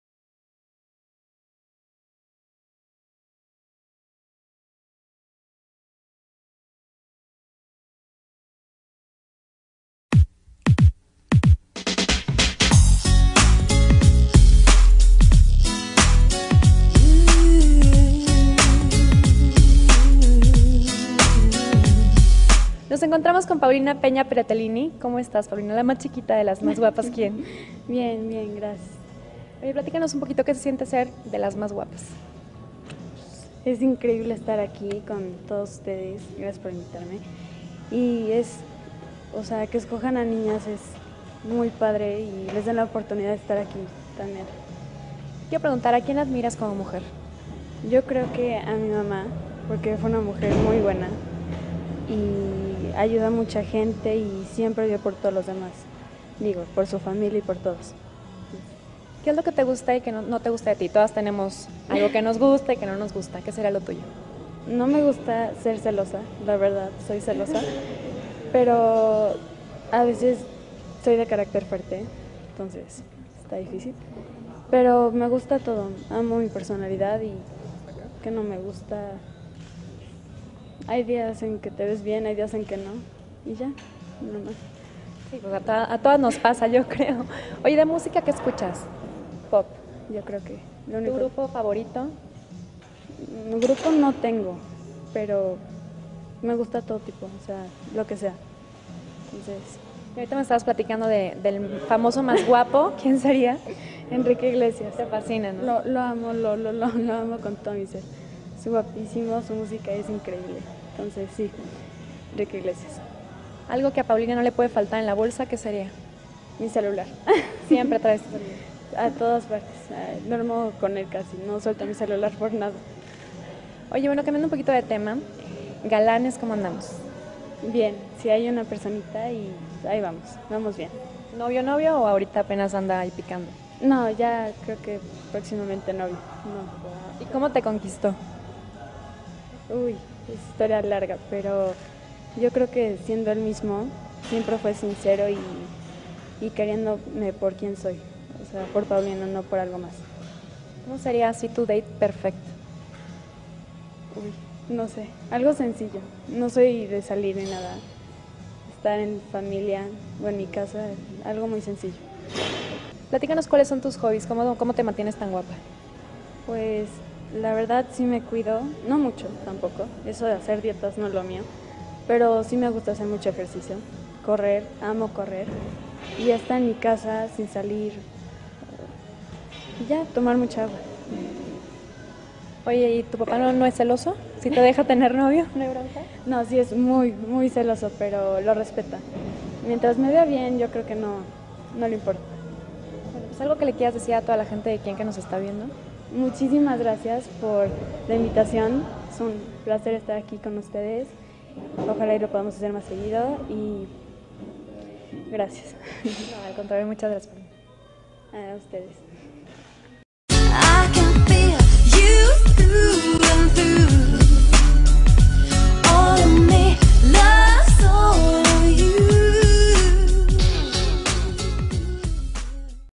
Entrevista a Paulina Peña Pretelini
En 2011, Paulina Peña fue una de nuestras 10 Niñas Guapas, ésta es la entrevista que se le hizo durante el photo shoot.